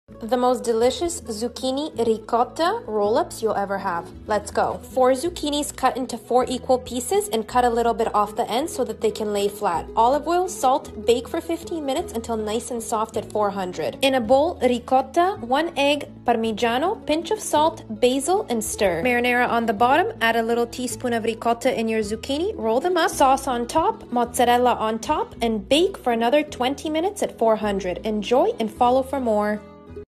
Moms zucchini ricotta lasagna roll-ups.
Moms zucchini ricotta lasagna roll-ups. These are everything you want in a dish but more. My favourite recipe of all time as I am a huge lover of zucchini.   makes 16 rolls but feel free to double it. You will need:
4 zucchini’s( palm size )
olive oil ( or any oil )
1/2 cup ricotta
1 small egg ( optional )
1/4 cup parmigiano
basil ( optional )
pinch of salt ( to taste )
marinara sauce ( 1 cup or as much as you like )
1/3 cup mozzarella ( as much as you like )
This method works incredible with a meat filling too
Pre-heat oven to 400F. Wash the zucchini , pat dry and cut the ends. Cut each one into 4 slices. The two end pieces we also cut from the back of each one so that they lay flat and are easy to roll. Don’t discard any peeled scraps as you can use them in your cooking for other recipes. Set them on a baking sheet and drizzle each side with olive oil plus a dash of salt. Bake until they are soft which will take you 15-20 min. Ours took 15 minutes. Take them out and let them cool a touch. In a separate bowl add the ricotta , parm , egg , dash of salt and basil and mix. Take a small pan ( we used 8 by 8 ) and add marinara sauce to cover the bottom ( we used our own homemade sugo which is on my highlights but you can use any store bought like rao’s) Take each zucchini with the wider side facing you and add about a teaspoon of ricotta to each one and roll them up. Add them all to your baking dish , top with more marinara and some mozzarella. Bake at 400F for 20 minutes or until nice and melted. Enjoy, and follow me for more.

best  recommendation for weight lose fast